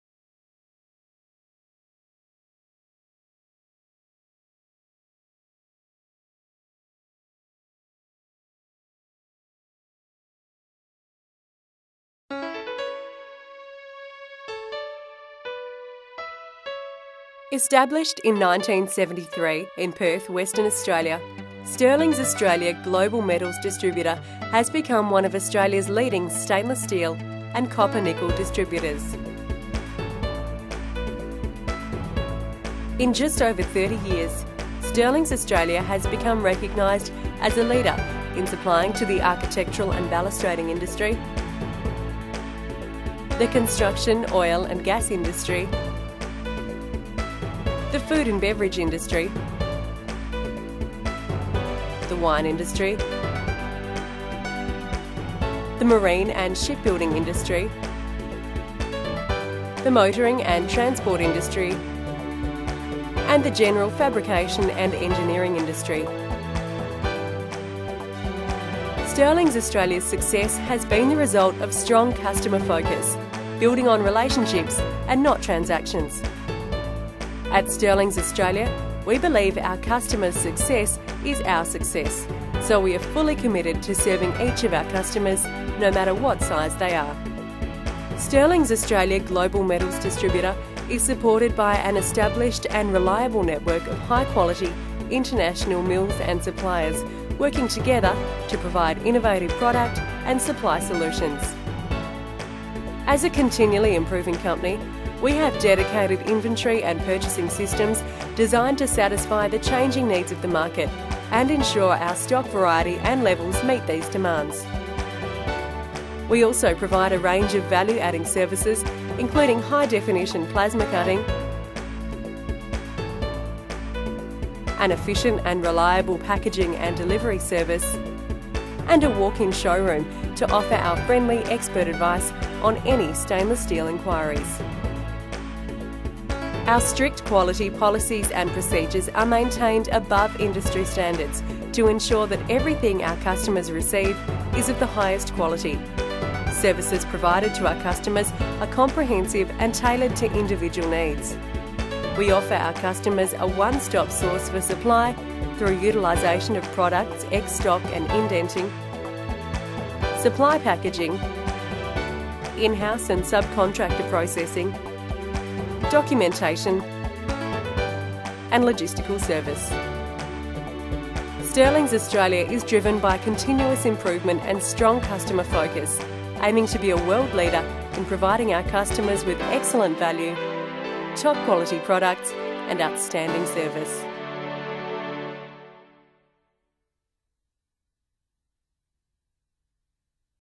Stirlings Australia Corporate Video
Stirlings Australia is a Stainless Steel and Copper Nickel supplier and stockist in Perth, Western Australia, driven by continuous improvement & strong customer focus, aiming to be a world leader in providing our customers with excellent value, top quality products & outstanding service.